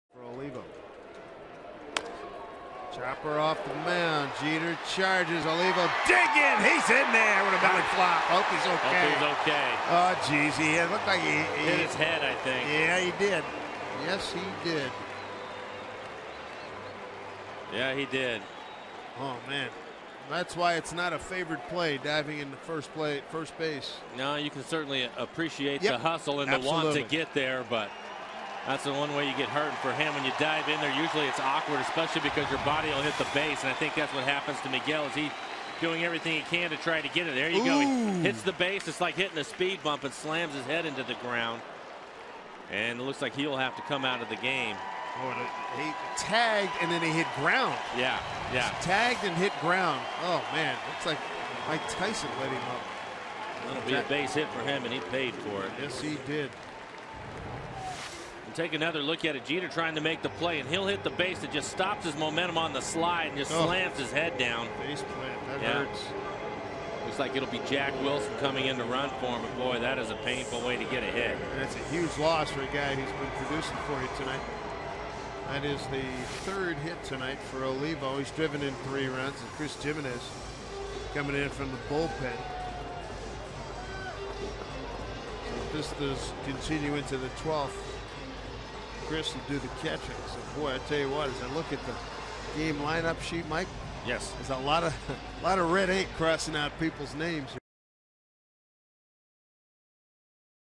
2011/05/28 Olivo's injury
NYY@SEA: Olivo is hurt sliding into first and leaves